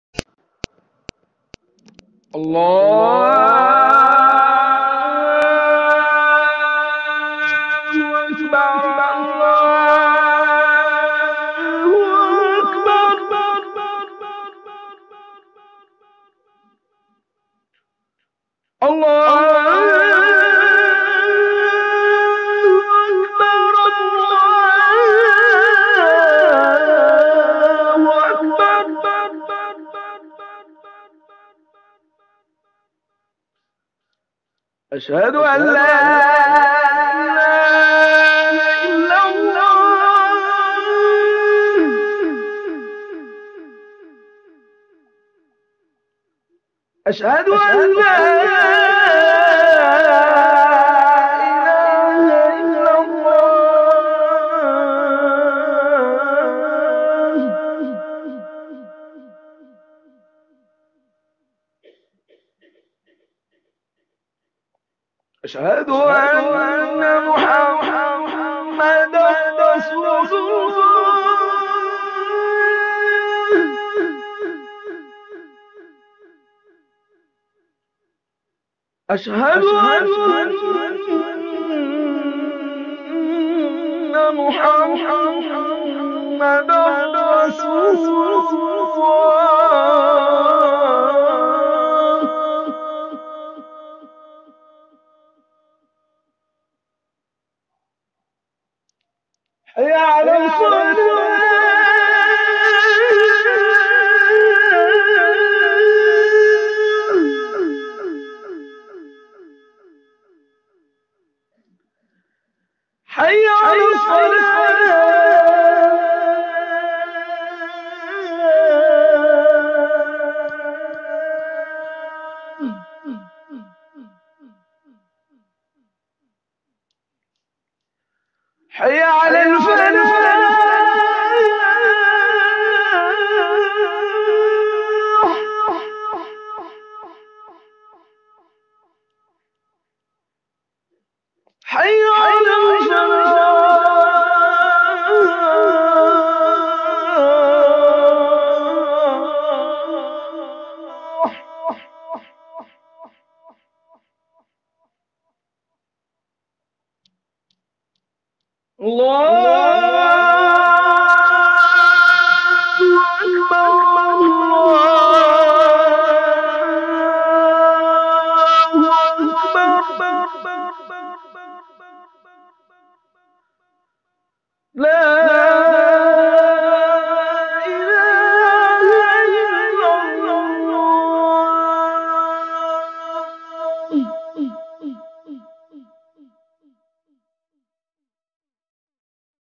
BEAUTIFUL AZAAN
Voice : qari Abu lubaba miftahi

(Moulana a.abdul aafi Hassan sab hussami)
Masjid e akbari
(akber Bagh.malakpet)

Editing by :: Hafiz Syed Zubair sab

Everyone listen this
And share with friends and relatives

(AL HADI TUBE)